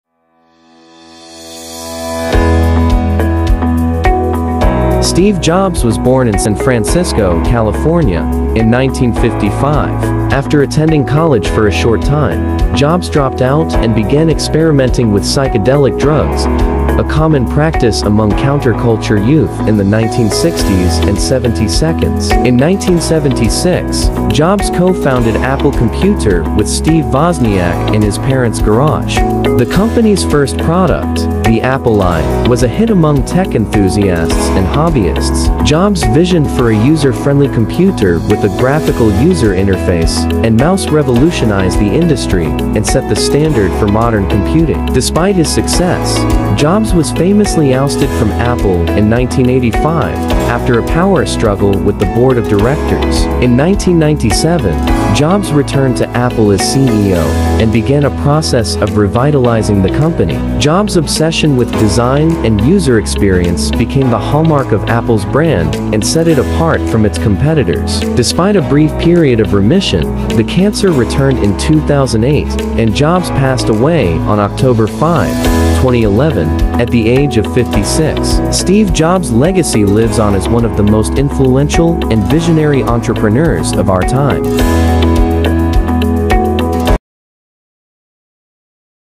From Dropout to Icon: The Inspiring Story of Steve Jobs
Steve Jobs is widely regarded as one of the most influential innovators and entrepreneurs of our time. However, his journey to success was not a straight line. In this video, we'll explore the fascinating story of Steve Jobs, from his humble beginnings as a college dropout to co-founding Apple and revolutionizing the tech industry.

We'll delve into the key moments, challenges, and triumphs that shaped Jobs' path and uncover the lessons we can all learn from his example. From his obsession with design and user experience to his uncompromising vision and leadership style, we'll reveal the secrets behind Jobs' success and legacy.